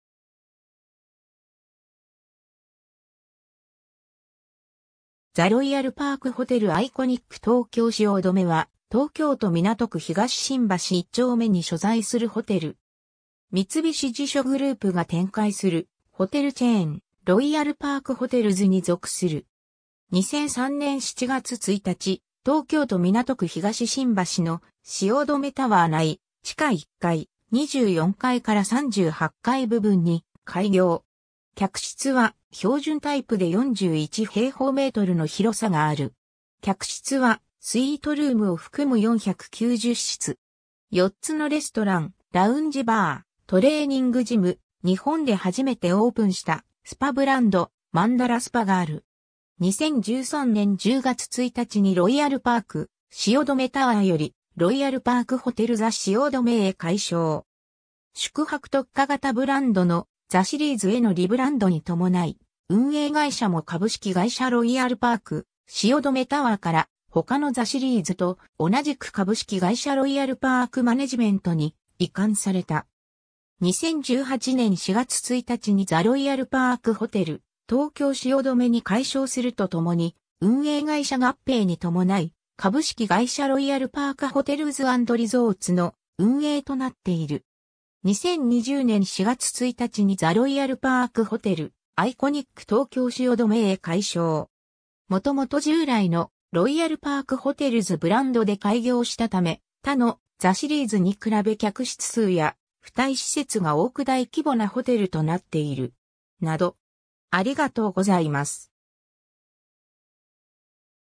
ザ ロイヤルパークホテル 東京汐留
ザ ロイヤルパークホテル アイコニック 東京汐留（ザ ロイヤルパークホテル アイコニック　とうきょうしおどめ、The Royal Park Hotel Iconic Tokyo Shiodome）は、東京都港区東新橋一丁目に所在するホテル。
三菱地所グループが展開するホテルチェーン「ロイヤルパークホテルズ」に属する。
 2003年7月1日、東京都港区東新橋（汐留シオサイト）の「汐留タワー」内地下1階、24階 - 38階部分に開業。
客室は標準タイプで41㎡の広さがある。
客室はスイートルームを含む490室。
4つのレストラン、ラウンジバー、トレーニングジム、日本で初めてオープンしたスパブランド「マンダラ・スパ」がある。
 2013年10月1日にロイヤルパーク汐留タワーよりロイヤルパークホテル ザ 汐留へ改称。
宿泊特化型ブランドの「THEシリーズ」へのリブランドに伴い、運営会社も株式会社ロイヤルパーク汐留タワーから他のTHEシリーズと同じく株式会社ロイヤルパークマネジメントに移管された。
 2018年4月1日にザ ロイヤルパークホテル 東京汐留に改称するとともに、運営会社合併に伴い、株式会社ロイヤルパークホテルズアンドリゾーツの運営となっている。
 2020年4月1日にザ ロイヤルパークホテル アイコニック 東京汐留へ改称。
 元々従来の「ロイヤルパークホテルズ」ブランドで開業したため、他の「THEシリーズ」に比べ客室数や付帯施設が多く大規模なホテルとなっている。
 など